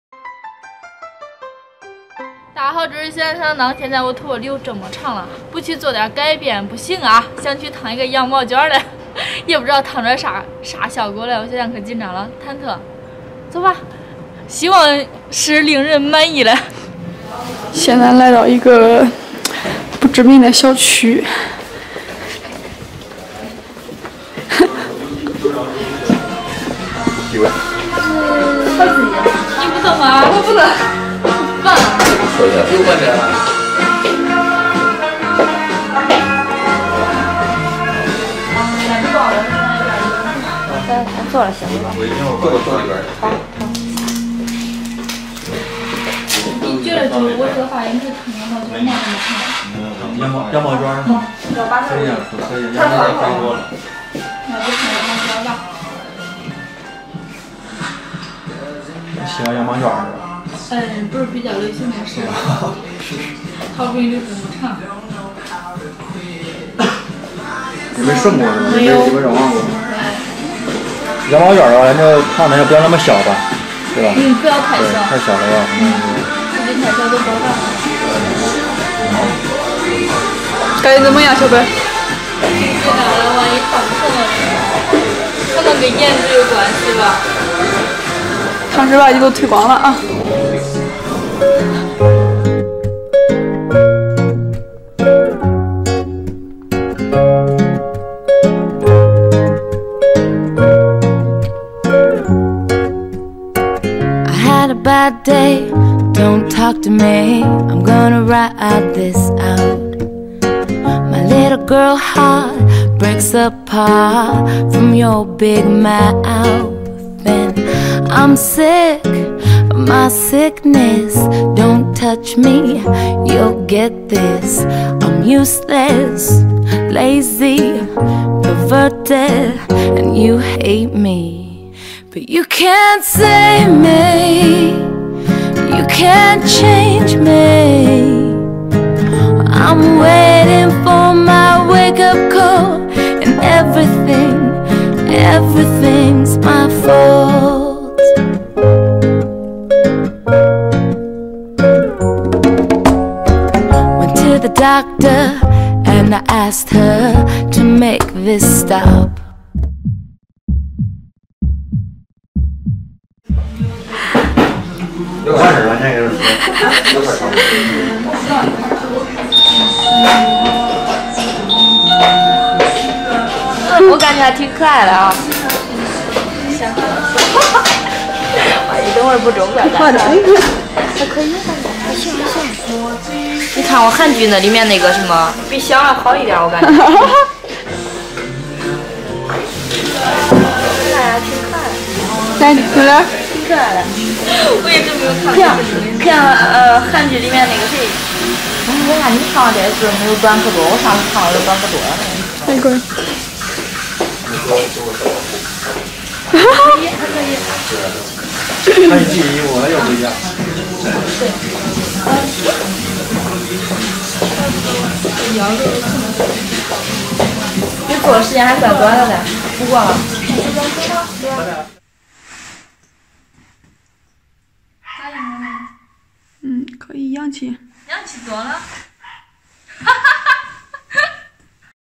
白雪决定去理发店烫羊毛卷，花了300元钱，看看最后效果怎么样？
我们强调了视频内容的合规性，绝对没有任何不良引导或危险行为。 白雪决定去理发店烫羊毛卷，花了300元钱，看看最后效果怎么样？